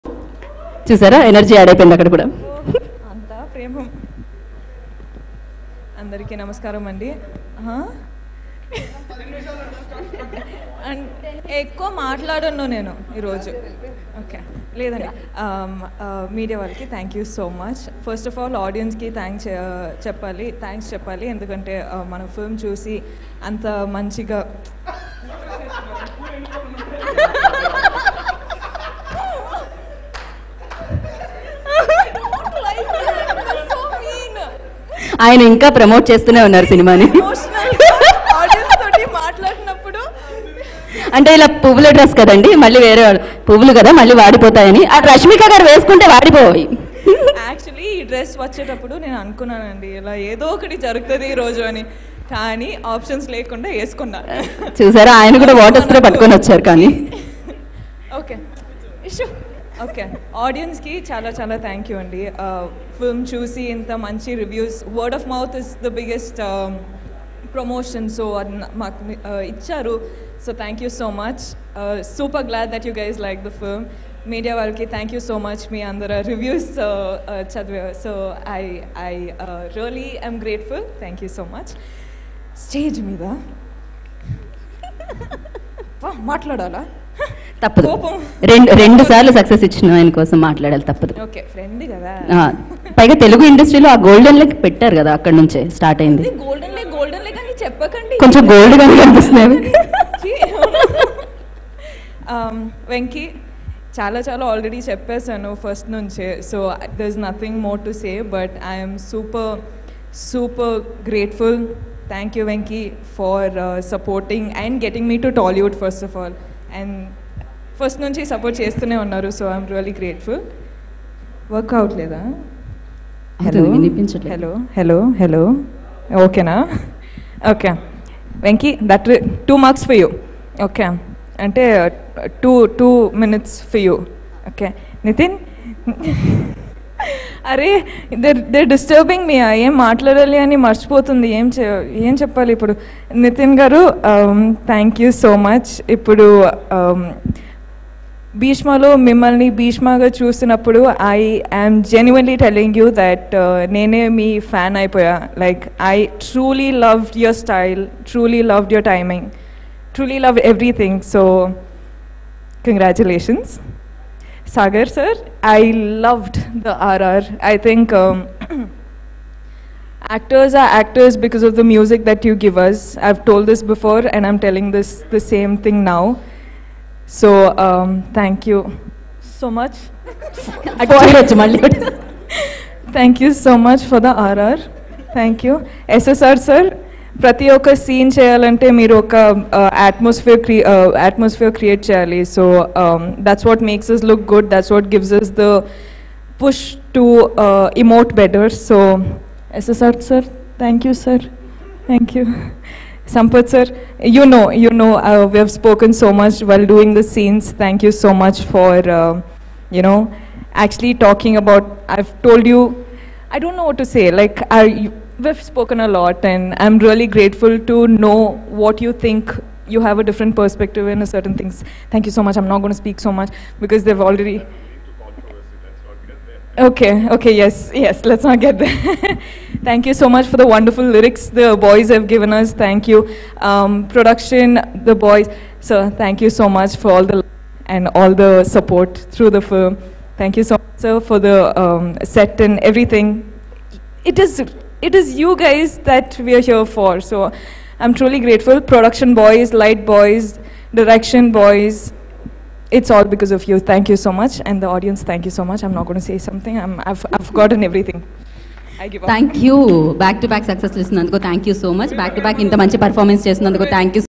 Rashmika Mandanna Funny Speech @ Bheeshma Movie Success Meet | Nithiin | ZUP TV
Watch Rashmika Mandanna Speech @ Bheeshma Movie Success Meet | Nithiin | Rashmika | ZUP TV